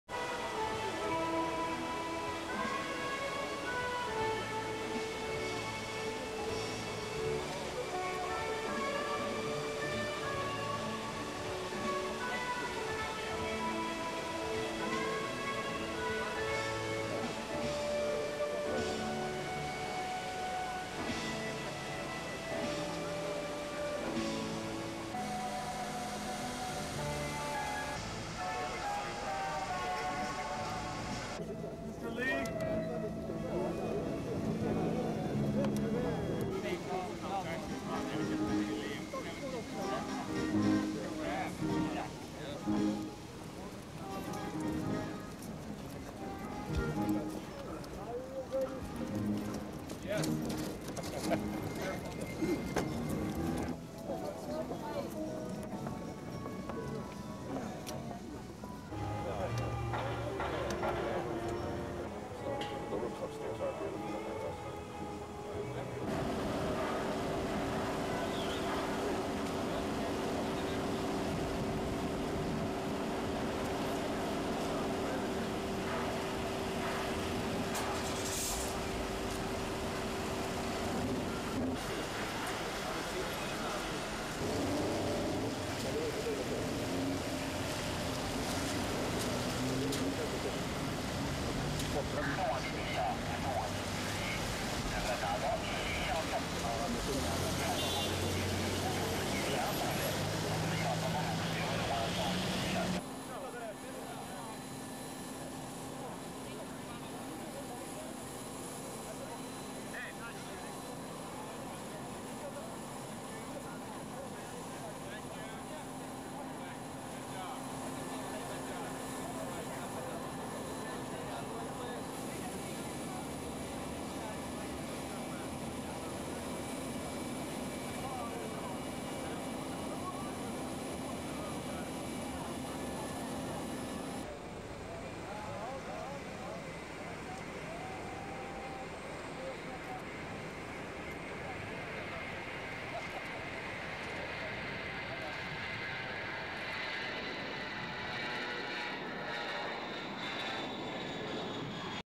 China-US Vice President Gore concludes his visit
(28 Mar 1997) T/I 10:00:30

US Vice President Al Gore left China for South Korea on Friday
(28/03), ending his landmark visit to boost Sino-American
relations. He was seen off from Shanghai by Vice Mayor Zhao
Qizheng, who he congratulated on the city's rapid economic
progress.

SHOWS:

SHANGHAI, CHINA, 28/03

WS people's square;
Vs of Gore with the mayor of Shanghai, Xu Kuangdi;
ws square;
workers;
Gore and Xu go into and coming out of the Shanghai cultural
Museum;
Diplomatic car drives off;
motorcade drives off;
Gore and wife at Shanghai airport departing;
waving goodbye;
plane takes off;

DUR: 2.32